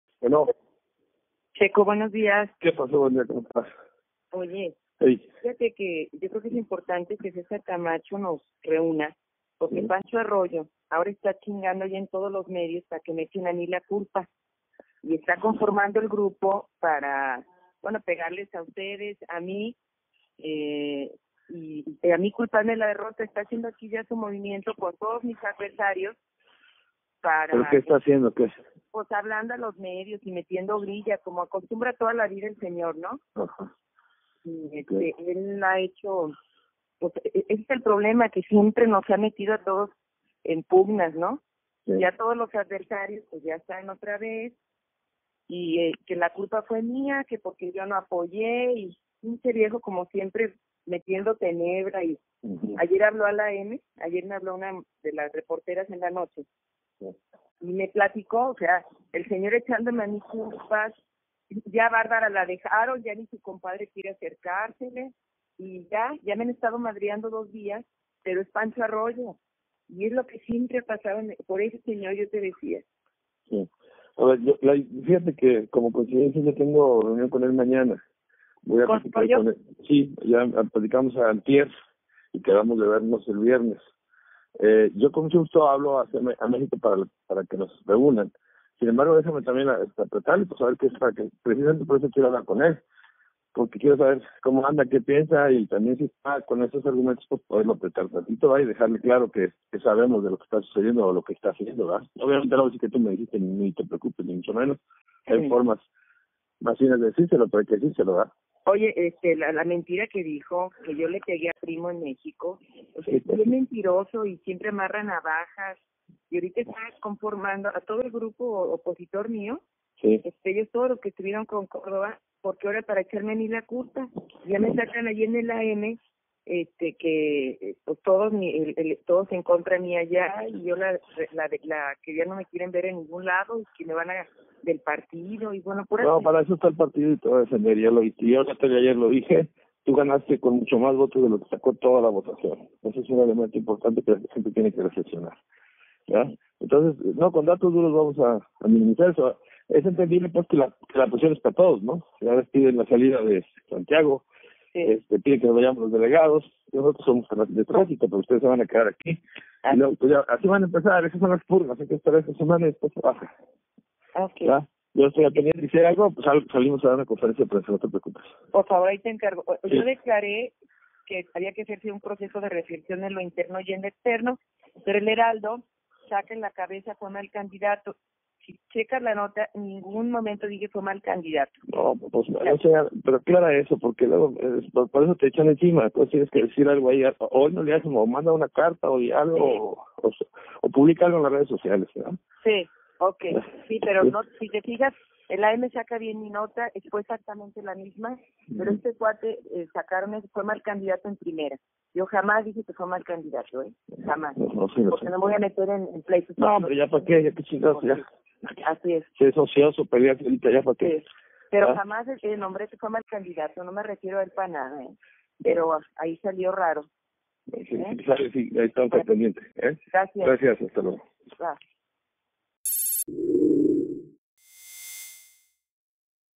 Conversación entre Barbara Botello y Sergio Marcelino Bravo
La exalcaldesa de León, llama al delegado del CEN del PRI en Guanajuato, para arremter contra el exdiputado federal Francisco Arroyo Vieyra, por haber hablado mal de ella, y señalarla por haber causado la derrota electoral en León.